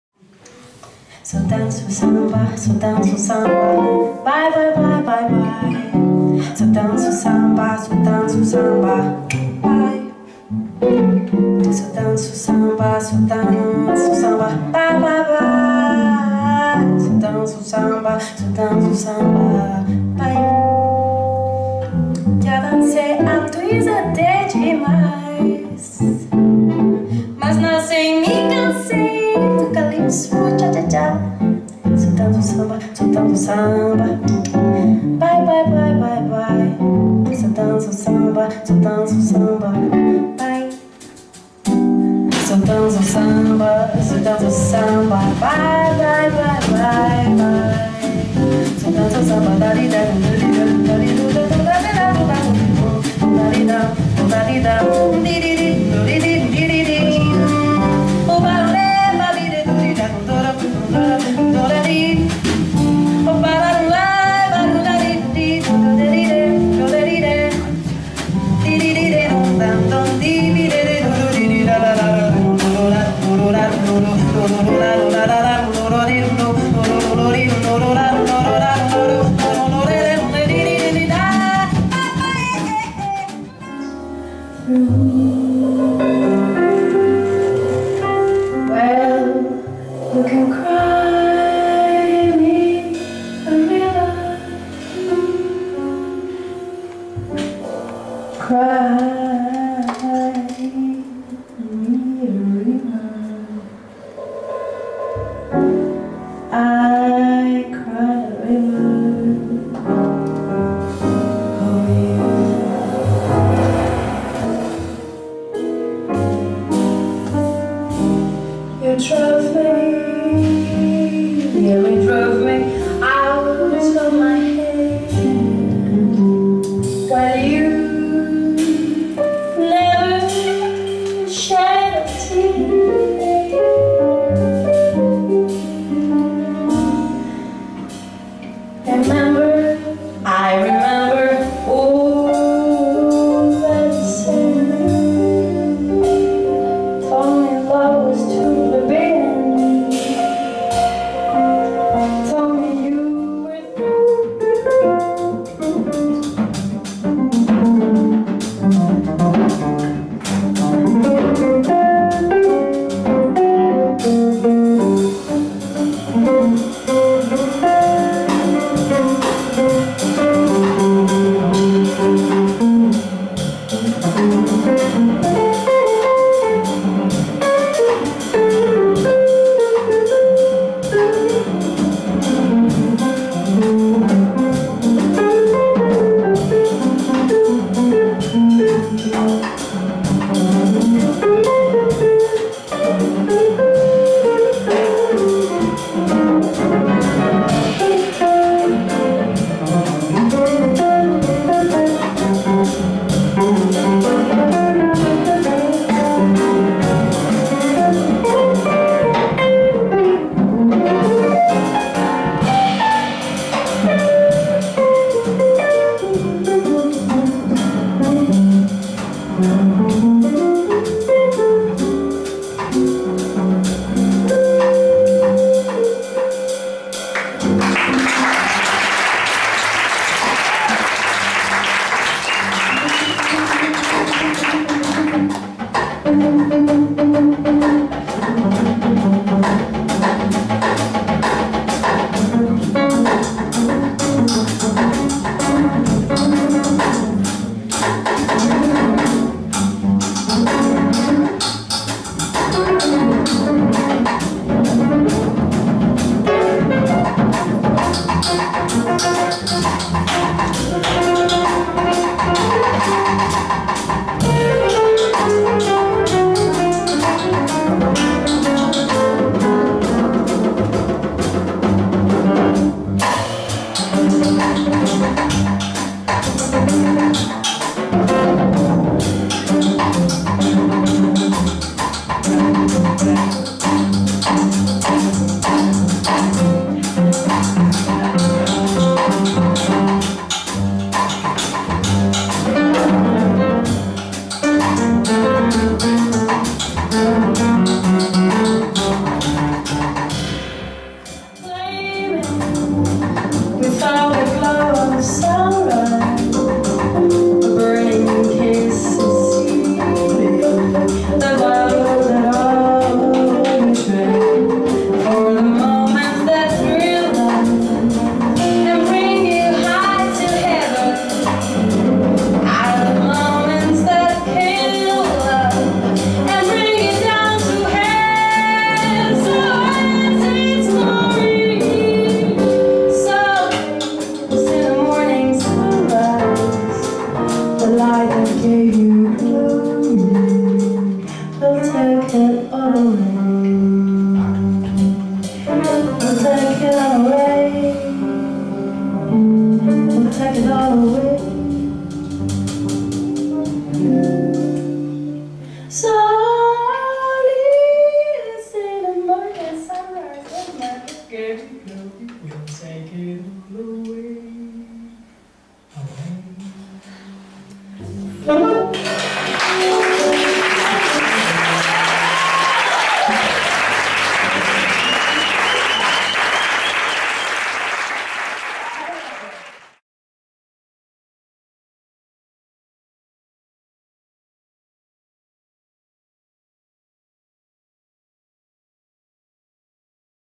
Concierto de profesores del 9º Seminario Improvisación Siero 2012 (2ª parte)
9º Seminario Improvisación Siero (Asturias), Julio 2012

Mapi Quintana - voz
Javier Rubio - saxo
Marco Martínez - guitarra
Jacobo de Miguel - piano
Horacio García - contrabajo
Jaime Moraga - batería